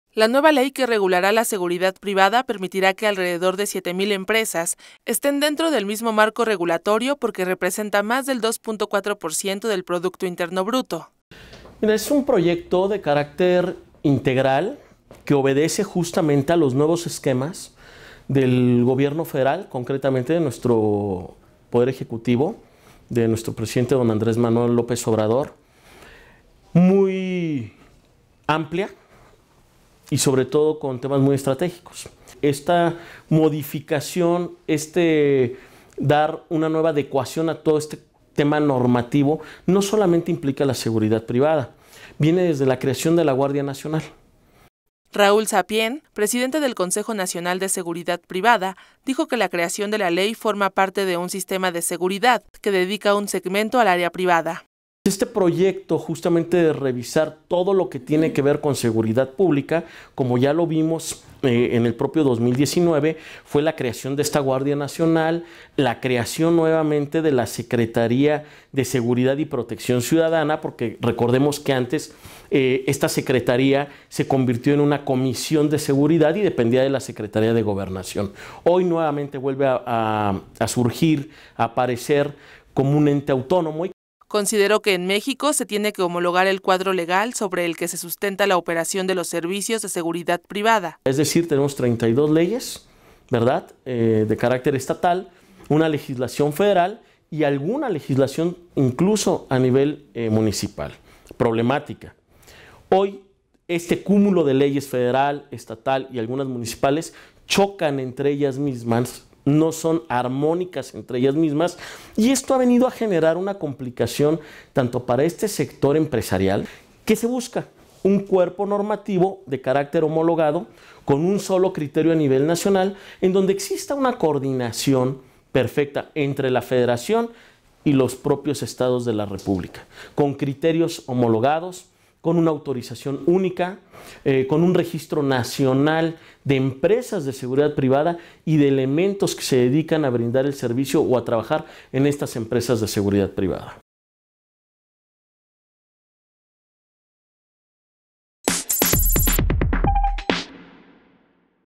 Nueva ley regulará la Seguridad Privada en México
La nueva Ley que regulará la Seguridad privada permitirá que alrededor de siete mil empresas estén dentro del mismo marco regulatorio porque representa más del 2.4 por ciento del Producto Interno Bruto